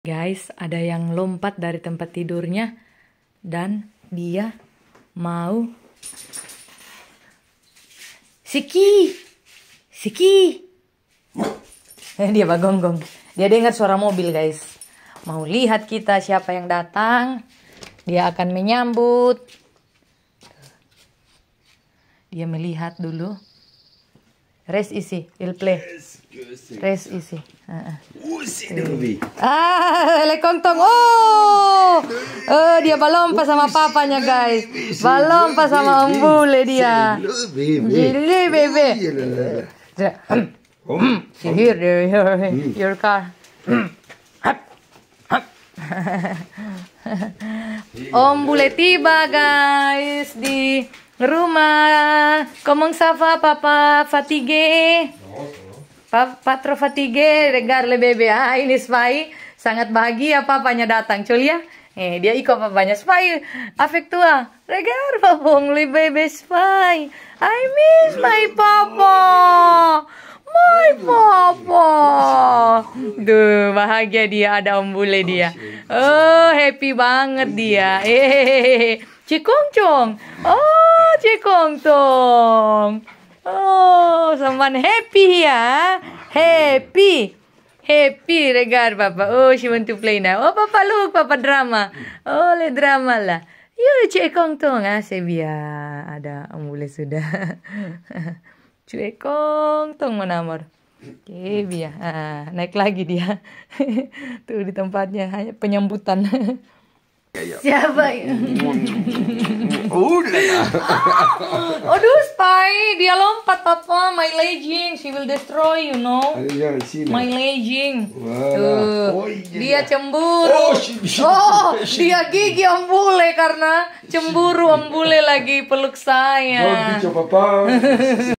Anjingku Spy Menyambut Om Bule
Anjingku Spy lompat dari tempat tidurnya dan menyambut om bule yang baru saja tiba.
Akhirnya sekarang sudah bersama lagi,setelah berpisah 4 hari.